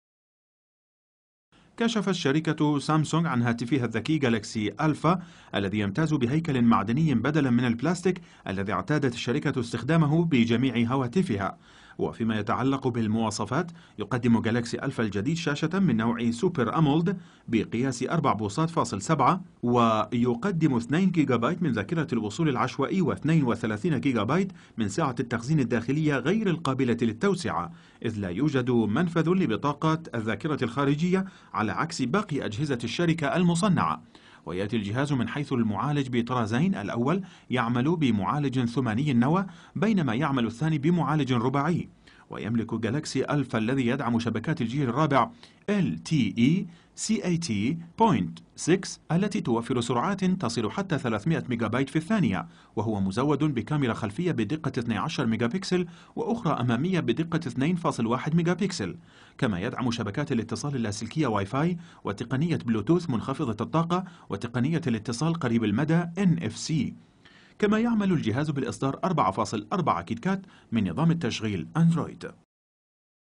نسخة جديدة من مجموعة غالاكسي الفا
سیجنال:
هاتف غالاكسي ألفا الجديد بهيكل معدني بدلًا من البلاستيك تلبية لطلبات السوق كشفت شركة سامسونغ عن هاتفها الذكي “غالاكسي ألفا”Galaxy Alpha الذي يمتاز بهيكل معدني بدلًا من البلاستيك الذي اعتادت الشركة استخدامه مع جميع هواتفها
تفاصيل على الرابط التالي :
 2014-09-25
قناة العالم الإخبارية